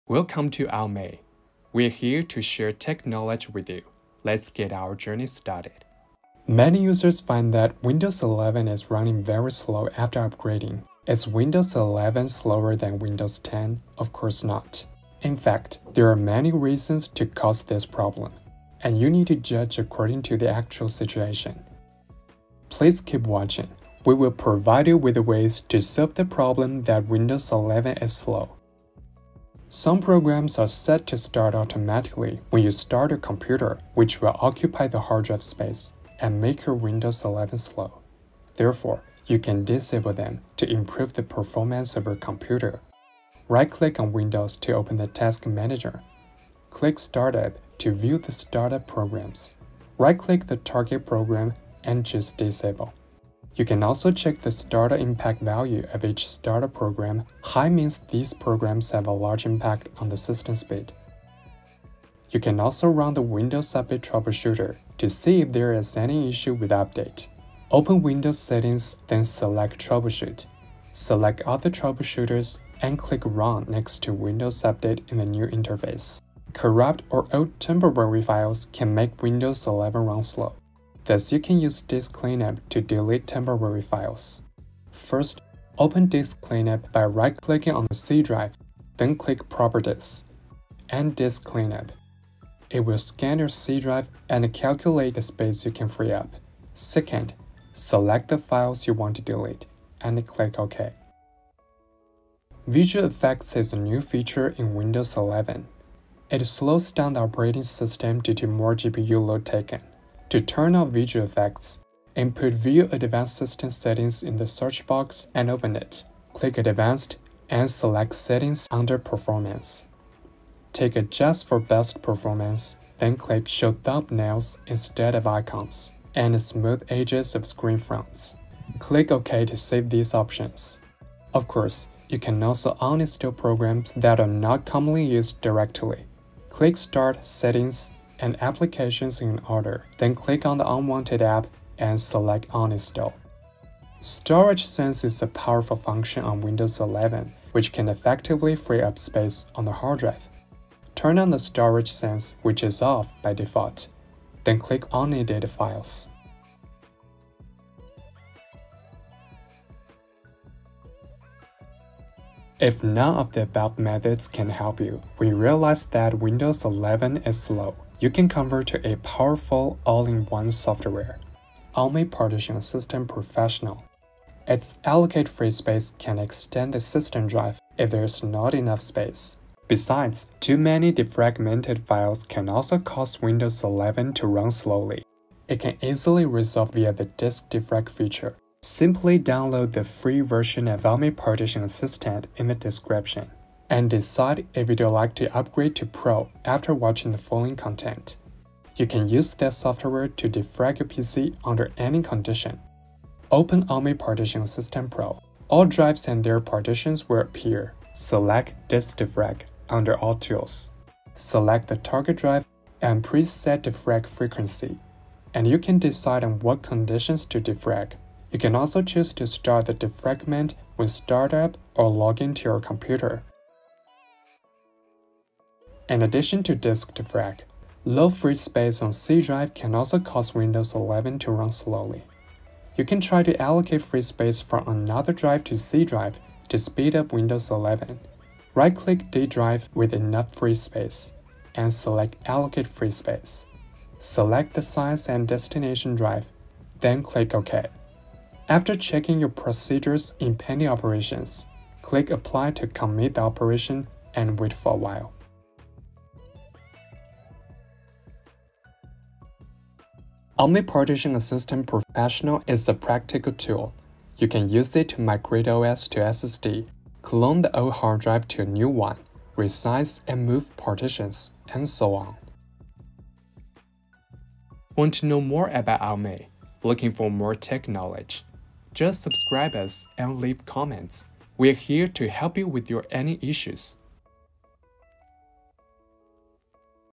Windows 11 Slow: Methods to Speed up Windows 11
✅ 【Windows 11 Slow】
⏰ 0:00 Start
⏰ 0:31 Method 1. Disable Startup Programs
⏰ 1:03 Method 2. Troubleshooting Windows 11
⏰ 1:17 Method 3. Delete Temporary Files
⏰ 1:43 Method 4. Disable Visual Effects
⏰ 2:12 Method 5. Uninstall Unwanted Programs
⏰ 2:25 Method 6. Storage Sense
⏰ 3:20 Option 1. Defragment the Hard Drive
⏰ 3:51 Option 2. Allocate Free Space

✅ 【Detail】
⏰ 0:31 Method 1. Disable Startup Programs
Some programs are set to start automatically when you start your computer which will occupy the hard drive space and make your Windows 11 slow. Therefore, you can disable them to improve the performance of your computer.

1. Right-click on Windows to open the "Task Manager", and click "Startup" to view the startup programs.
2. Right-click the target program and choose "Disable".
You can also check the "Startup Impact" value of each startup program, "High" means that these programs have a large impact on the system speed.

⏰ 1:03 Method 2. Troubleshooting Windows 11
You can also run the Windows Update troubleshooter to see if there is any issue with the update.
1. Open Windows Settings, then select "Troubleshoot".
2. Select other troubleshooters and click "Run" next to "Windows Update" in the new interface.

⏰ 1:17 Method 3. Delete Temporary Files
Corrupt or old temporary files can make Windows 11 run slow. Thus, you can use Disk Cleanup to delete temporary files.

First, open Disk Cleanup by right-clicking on the C drive, then click "Properties" and "Disk Cleanup". It will scan your C drive and calculate the space you can free up. Second, select the files you want to delete and click "OK" in the interface.

⏰ 1:43 Method 4. Disable Visual Effects
Visual effects is a new feature in Windows 11. It slows down the operating system due to more GPU load taken. To turn off visual effects:

1. Input "View advanced system settings" in the search box and open it.
2. Click "Advanced" and select "Settings" under "Performance".
3. Tick "Adjust for best performance", then tick "Show thumbnails instead of icons" and "Smooth edges of screen fonts".
4. Click "OK" to save these options.

⏰ 2:12 Method 5. Uninstall Unwanted Programs
Of course, you can also uninstall programs that are not commonly used directly. Click "Start", "Settings" and "Applications" in order. Then, click on the unwanted app and select "Uninstall".

⏰ 2:25 Method 6. Storage Sense
Storage Sense is a powerful function on Windows 11, which can effectively free up space on the hard drive. Turn on the Storage Sense which is off by default, then click unneeded files.

⏰ 3:20 Option 1. Defragment the Hard Drive
You can use this software to defrag your PC under any condition.
1. Open AOMEI Partition Assistant Pro, all drives and their partitions will appear, select "Disk Defrag" under "All Tools".
2. Select the target drive and preset defrag frequency, and you can decide on what conditions to defrag.
3. You can also choose to start the defragment when startup or login to your computer.

⏰ 3:51 Option 2. Allocate Free Space
In addition to disk defrag low free space on the C drive can also cause Windows 11 to run slowly. You can try to allocate free space from another drive to C to speed up Windows 11. The detailed steps are as follows:

1. Open AOMEI Partition Assistant, right-click drive D with enough free space and select "Allocate Free Space".
2. Select the size and destination drive, then click "OK".
3. After checking your procedures in "Pending Operations", click "Apply" to commit the operation and wait for a while.